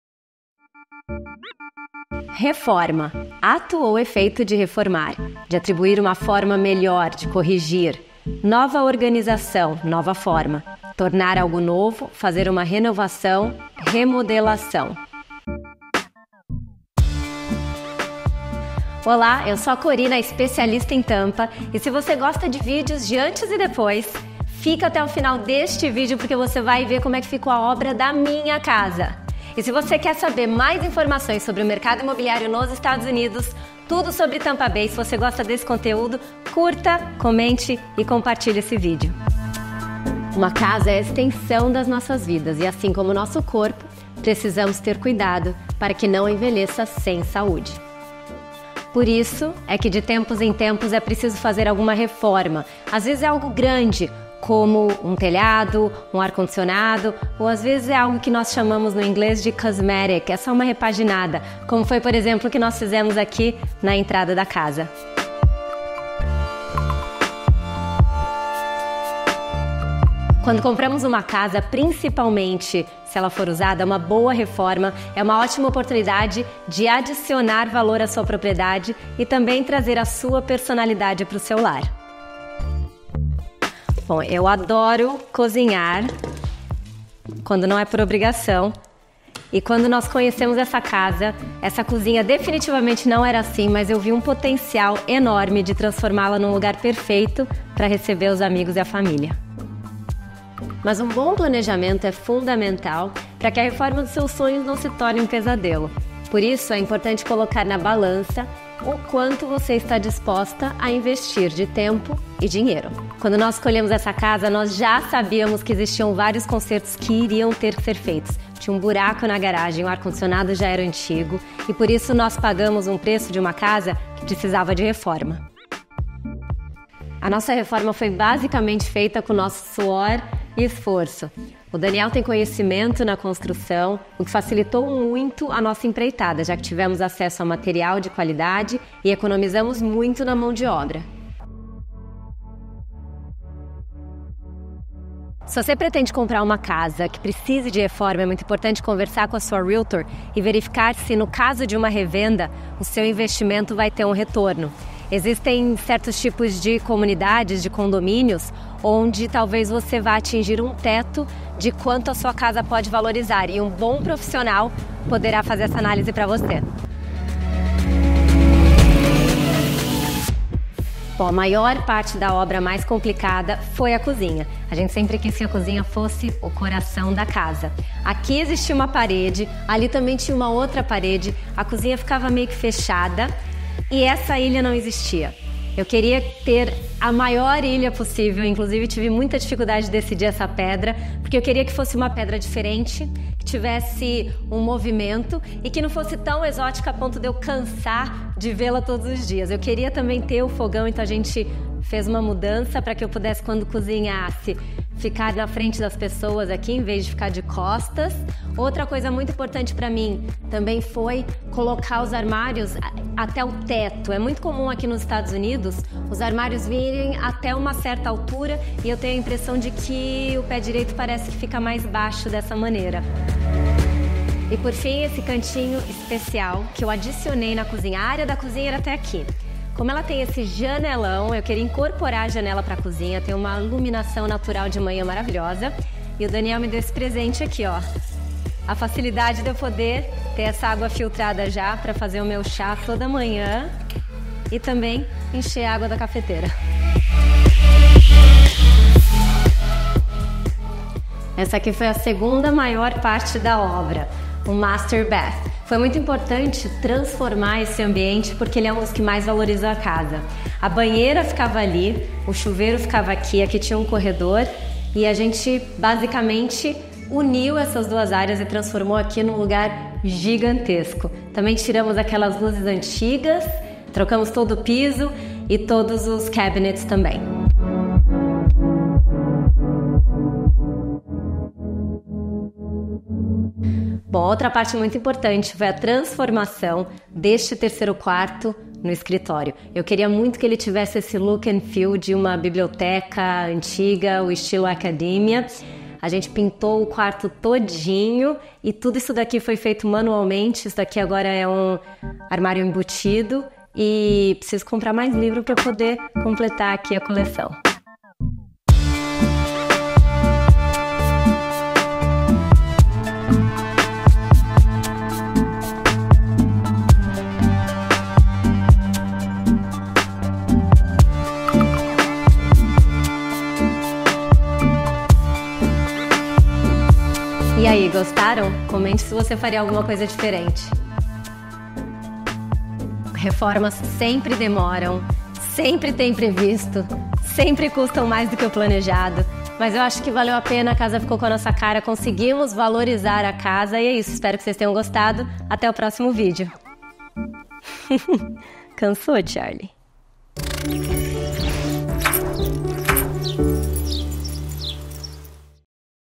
361. Tour pela Casa de um Realtor na Flórida: Antes e Depois da Reforma!
Esta semana, estamos saindo um pouco da nossa programação habitual para explorar algo muito especial.

Você já se perguntou como é a casa de um corretor imobiliário na Flórida? Bem, hoje você terá a resposta!

A Corina e o Daniel gentilmente nos recebem de braços abertos para compartilhar a jornada de reformarem a casa deles com as próprias mãos, todos os desafios dessa transformação que teve desde mudanças estruturais a pequenos toques de design.

Não perca a chance de ver o antes e depois dessa casa deslumbrante. Esta é uma história de inspiração e determinação que você não vai querer perder!

Comente aqui embaixo o que achou dessa reforma e temas que você gostaria de ver aqui em nosso canal.

Para conversar com a Corina, Marga ou algum dos nossos especialistas, entre em contato através via WhatsApp ou por e-mail

Corina: @corinalessasilva
Marga: @margawelterrealtor
@TAMPA BAY KEY